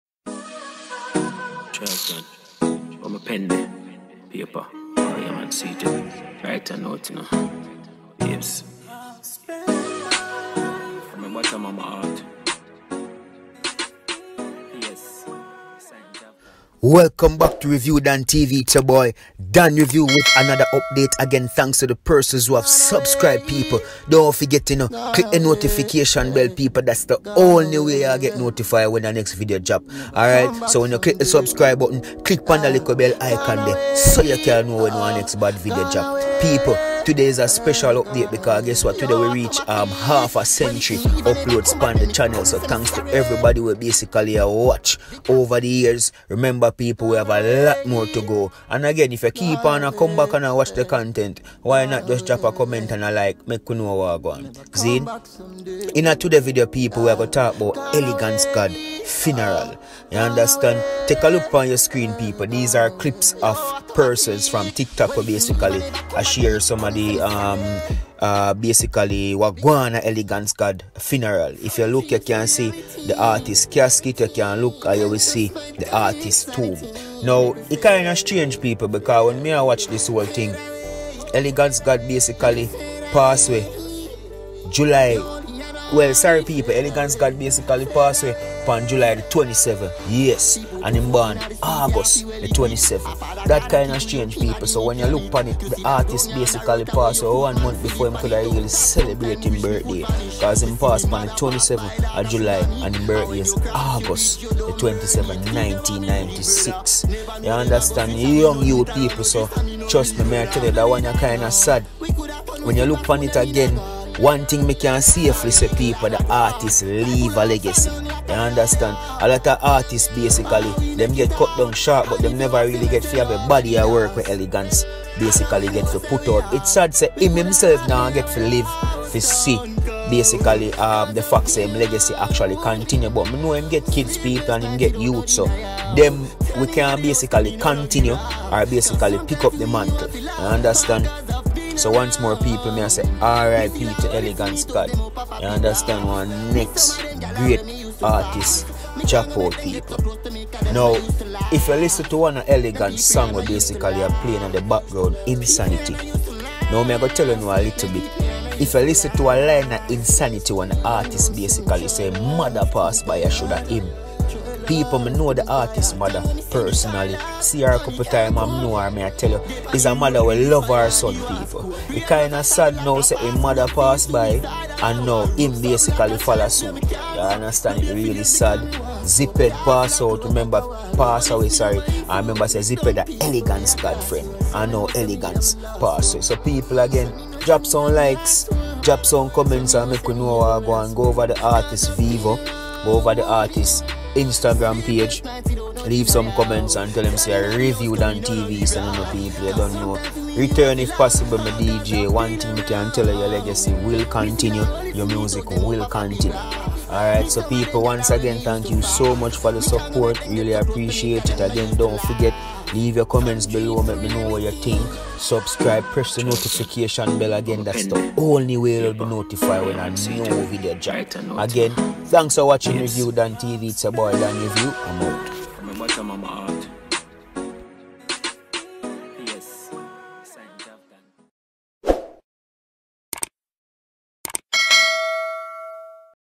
Elegance Gad - Funeral ( What happened to his mom !? Was it a Spiritual calling Born : 27 Died : 27
This video speaks about elegance Funeral and also the fact the he was born on the 27th and died on the 27th one month before his birthday